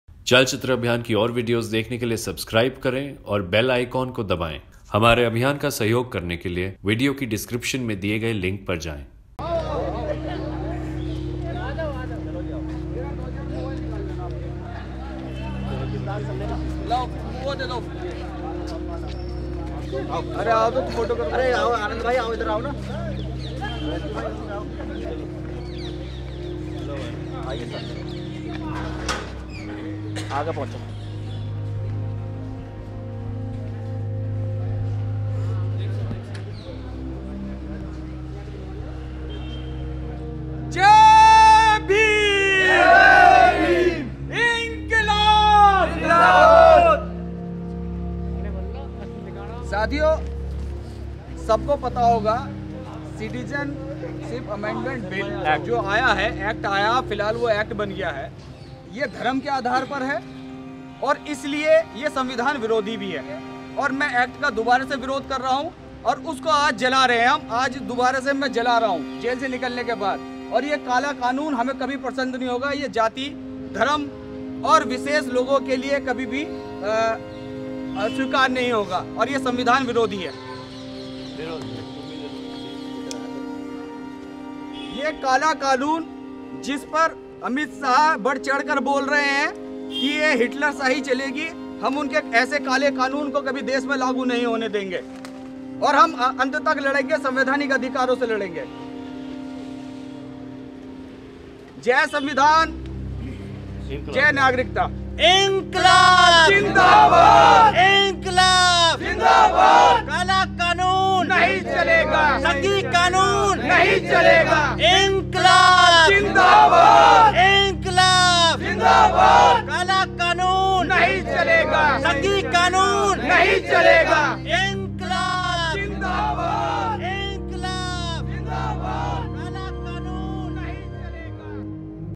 मेरठ में काला कानून (CAA) को दलितों ने जलाया | Dalits burn down Citizenship Amendment Act in Meerut
मेरठ के दलित लीडर डॉ सुशिल गौतम ने CAA का विरोध किया जिस कारण उन्हें पुलिस ने 3 दिन के गिरफ़तार कर लिया| जेल से निकलने के बाद उन्होंने ब्लू पैंथर के अन्य साथियों के साथ CAA कानून की कॉपी को जला दिया | इस समय वह भूमिगत हैं और उनके परिवार को पुलिस द्वारा परेशां किया जा रहा है|
 टीम : नकुल सिंह  साव्हने,  शाकिब यामीन रअंग्रेज़ज
Dalit leader from Meerut, Dr. Sushil Gautam was arrested for 3 days in Meerut for protesting against CAA. After Sushil was released, he along with other activists from Blue Panthers burnt down copies of CAA saying this is the second Manu Smriti and deserves to be burnt down the way Baba Saheb burnt down the first one. Sushil is currently in hiding. Meerut police is harassing his family and even illegally detained his father for a few hours.
Crew: Nakul Singh Sawhney and Shakib Yameen Rangrez